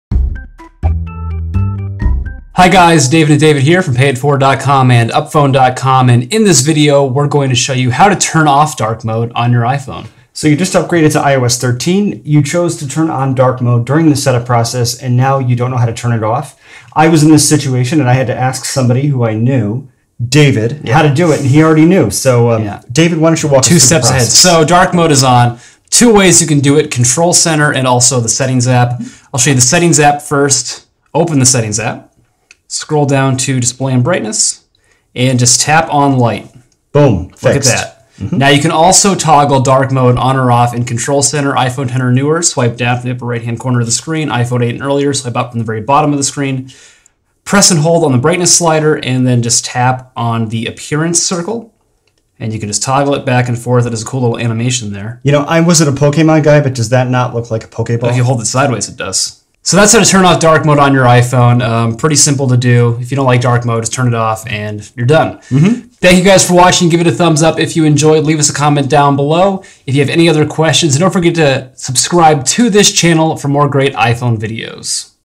How To Turn Off Dark Mode on iPhone in iOS 13
Two Apple experts show you how to turn off Dark Mode on your iPhone. Dark Mode is the light-on-dark color scheme that you can set up on your iPhone after installing iOS 13.

If you decide you want to turn it off, open Settings and tap Display & Brightness. Then, tap Light at the top of the screen.

You can also turn off Dark Mode in Control Center. Force touch the Brightness slider, then tap on the Appearance button in the lower left-hand corner of the screen.